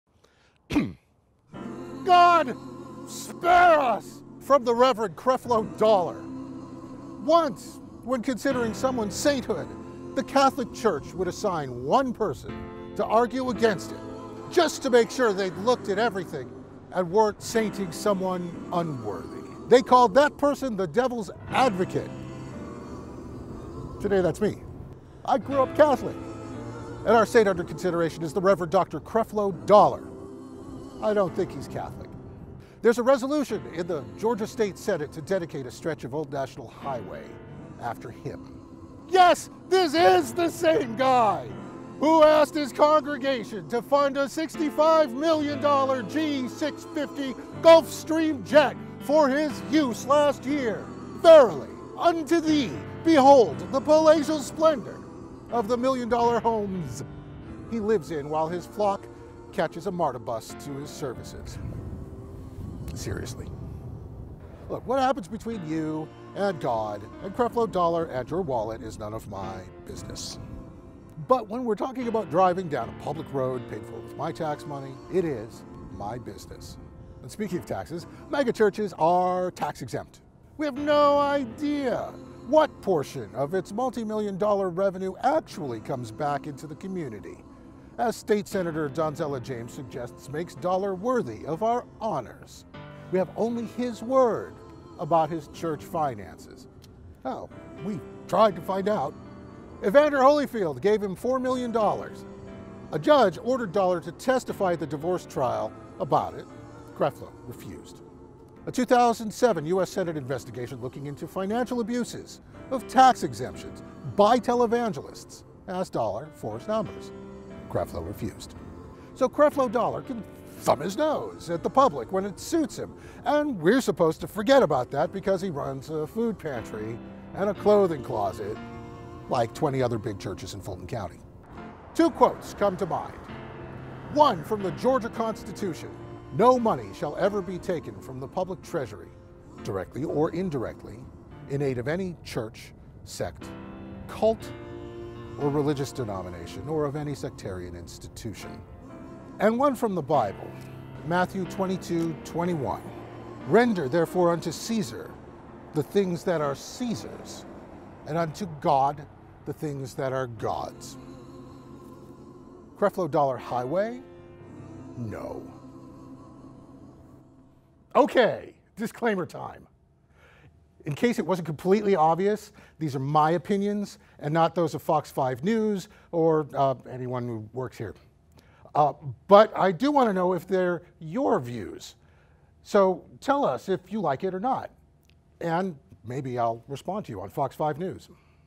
OPINION: God, Spare Us from Creflo Dollar
“God, Spare Us from Creflo Dollar” (by contributor George Chidi.)

OPINION: Once, when considering someone’s sainthood, the Catholic Church would assign one person to argue against it, just to make sure they’d looked at everything and weren’t sainting someone unworthy.

They called that person the devil’s advocate. Today… that’s me.

I grew up Catholic. And our saint is the Rev. Dr. Creflo Dollar. I don’t think he’s Catholic.

There’s a resolution in the Georgia State Senate to dedicate a stretch of Old National Highway after him.

YES! This IS the same guy who asked his congregation to fund a $65 million G650 Gulfstream jet for his use last year. VERILY UNTO THEE, behold the palatial splendor of the million-dollar homes he lives in while his flock catches a MARTA bus to his services.

Seriously.

What happens between you, God, Creflo Dollar and your wallet is none of my business. But when we’re talking about driving down a public road paid for with my tax money, it is my business.

And speaking of taxes, megachurches are tax exempt. We have no idea what portion of its multi-million dollar revenue actually comes back into the community, as State Sen. Donzella James suggests makes Dollar worthy of our honors. We have only his word about church finances.

Oh, we’ve tried to find out. Evander Holyfield gave him $4 million. A judge ordered Dollar to testify in Holyfield’s divorce trial about it. Creflo refused. A 2007 U.S. Senate investigation looking into financial abuses of tax exemptions by televangelists asked Dollar for his numbers. Creflo refused.

So, Creflo Dollar can thumb his nose at the public when it suits him. And we’re supposed to forget about that because he runs a food pantry and a clothing closet like 20 other big churches in Fulton County.

Two quotes come to mind. One from the Georgia Constitution: "No money shall ever be taken from the public treasury, directly or indirectly, in aid of any church, sect, cult, or religious denomination or of any sectarian institution."

And one from the Bible. Matthew 22:21 "Render unto Caesar the things that are Caesar's, and unto God the things that are God's."

Creflo Dollar Highway? No.

(This “Like it or Not” editorial is a statement of opinion from George Chidi. The views expressed are not necessarily those of FOX 5 Atlanta or its employees.)

Click here to view the full response from World Changers Church International